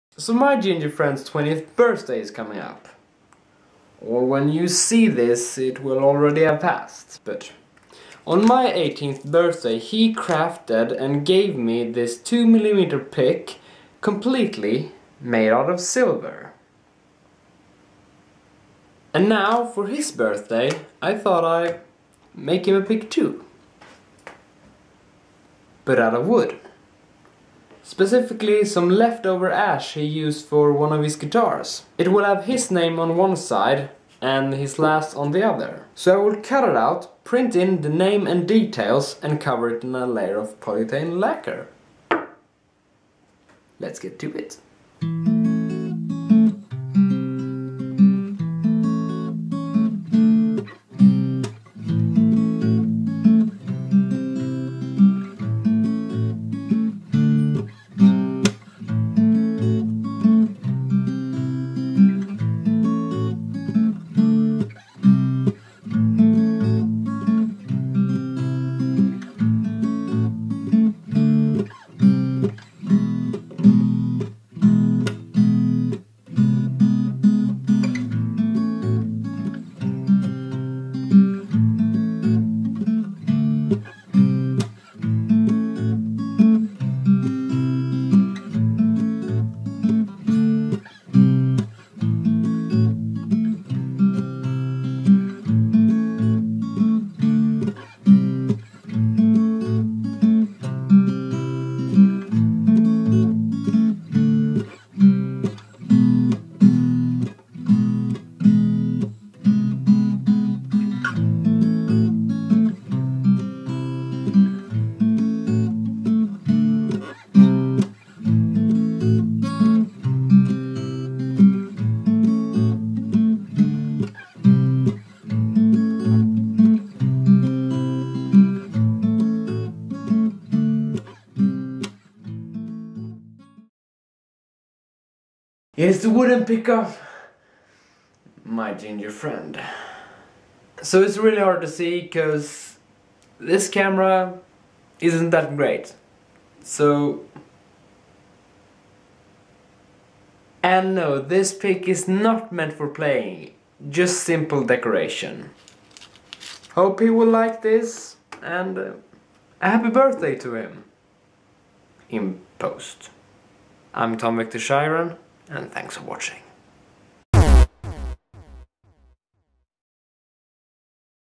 Handmade Wooden Guitar Pick
My ginger friends birthday was just around the corner and since he made me a pick out of silver, Im made one for him out of wood.